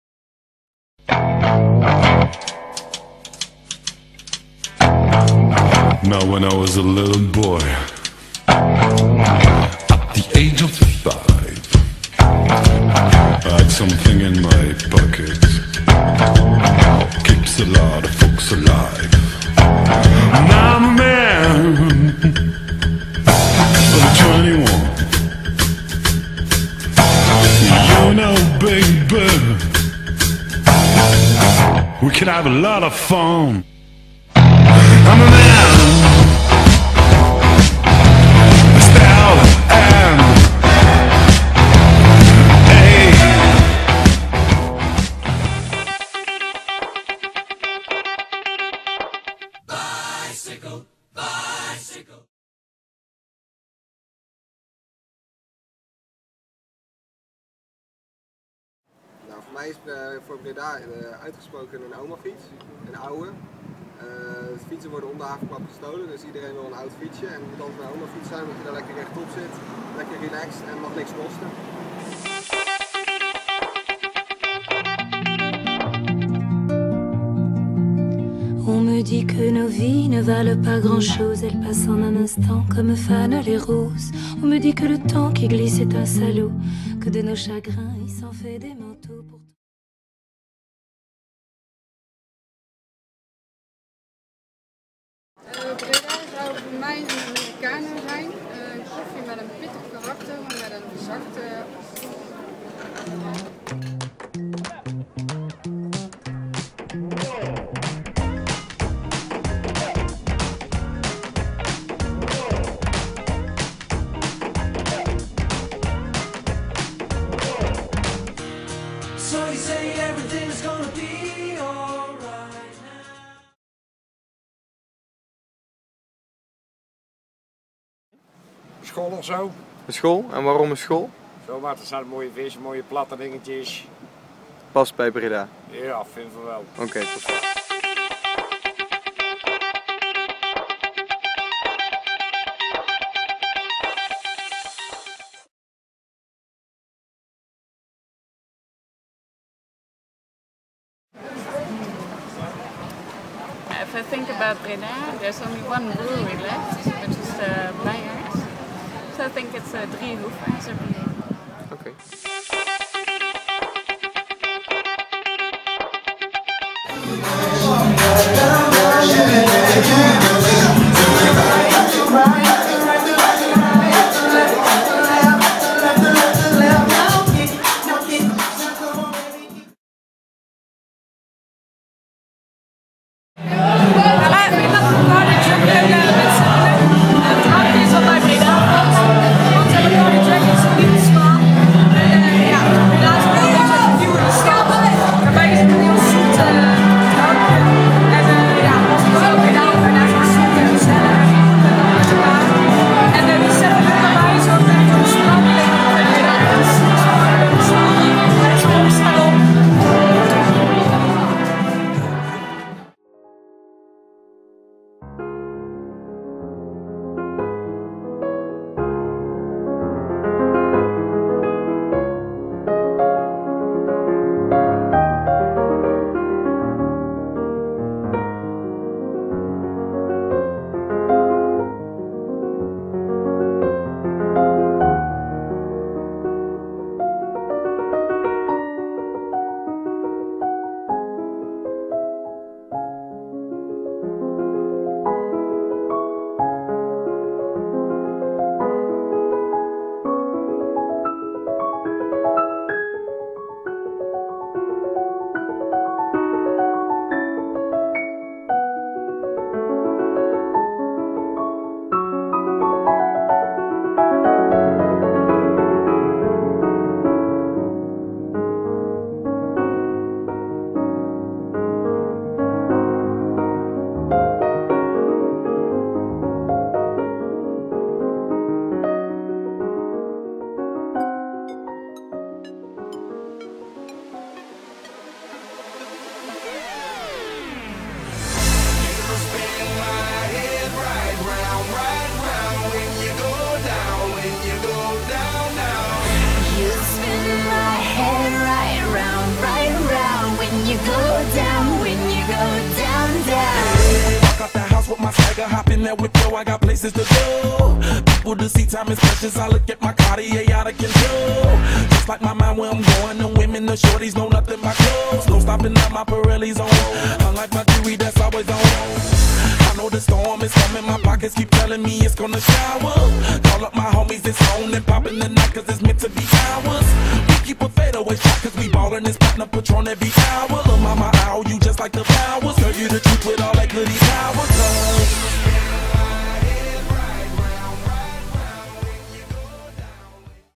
Breda, could you imagine?
A one day assignment, where the students had to make a trailer to promote Breda in a Imagineering way.